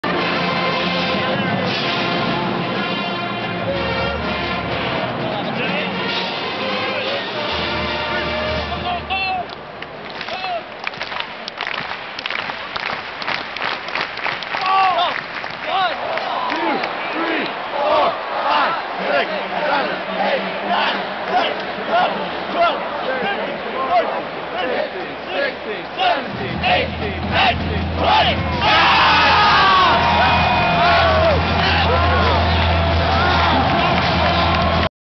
Johnny doing push ups at Gopher Game
Every time the gophers score Goldy does push ups for every point scored... well so does our buddy Johnny